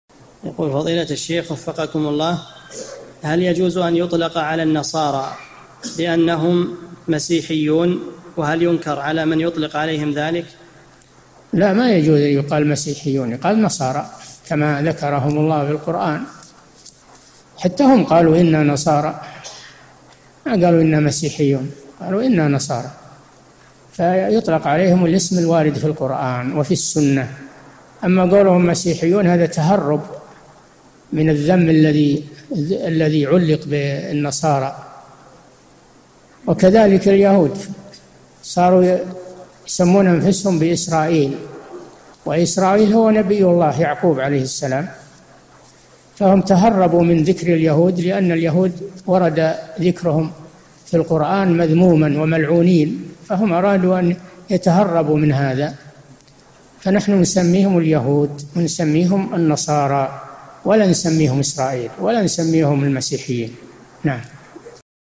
[838 /1350] هل يطلق على النصارى بأنهم مسيحيون؟ وهل ينكر على من يطلق عليهم ذلك؟ الشيخ صالح الفوزان
-[838 /1350] السؤال: هل يجوز أن يطلق على النصارى بأنهم مسيحيون؟ وهل ينكر على من يطلق عليهم ذلك؟
 الجواب: لا، ما يجوز أن يُقال: مسيحيون، يُقال: نصارى، كما ذكرهم الله في القرآن، حَتَّىٰ هم قالوا: إنَّا نصارى، ما قالوا: إنَّا مسيحيون، قالوا: إنَّا نصارى، فيُطلق عليهم الاسم الوارد في القرآن وفي السنَّة، أمَّا قولهم: مسيحيون؛ هٰذَا تهرُّب من الذنب الَّذِي عُلِّق بالنصارى، وكذلك اليهود صاروا يسمون أنفسهم بـ"إسرائيل"، وإسرائيل هو نبي الله يعقوب عليه السلام ، فهم تهربوا من ذِكر اليهود؛ لأنَّ اليهود ورد ذكرهم في القرآن مذمومًا وملعونين، فهم أرادوا أن يتهربوا من هٰذَا؛ فنحن نسميهم اليهود، ونسميهم النصارى، ولا نسميهم إسرائيل، ولا نسميهم المسيحيين.
للاشتراك في قناة الشيخ صالح الفوزان
أسئلة وأجوبة من شرح قاعدة جليلة ( 1350 ) مقطع

للاستفسار والإقتراحات والدعم
رئيس الفريق
لمشروع كبار العلماء